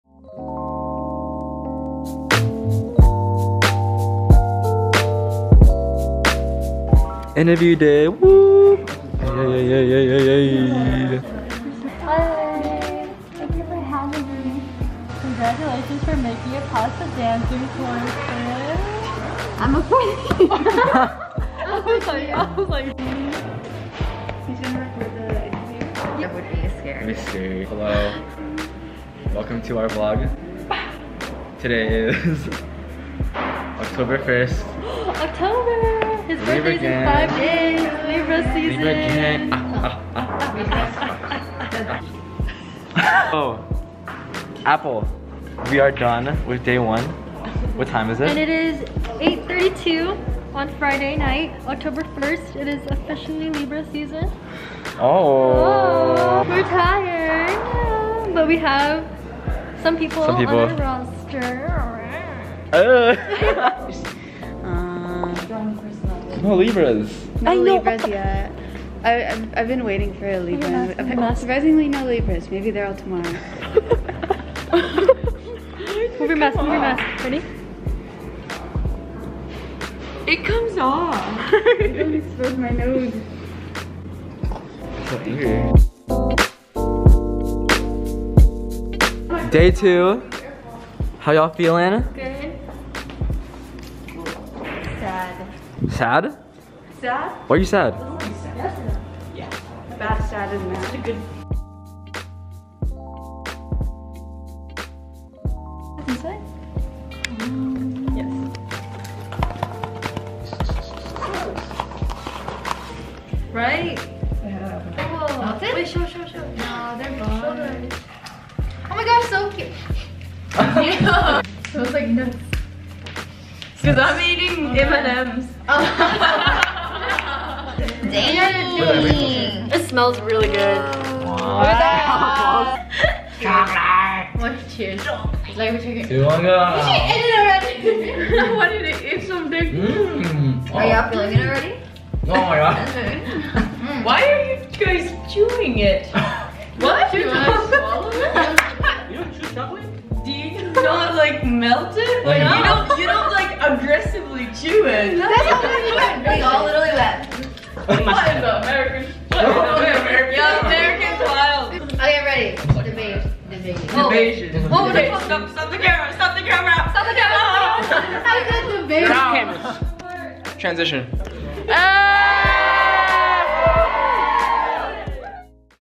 909 Gen 25 Interview Vlog + Mukbang?? + Gen 25 Reveal?
Hello! This is just a vlog of the shenanigans we did during the interviews lol.

-Wilson :)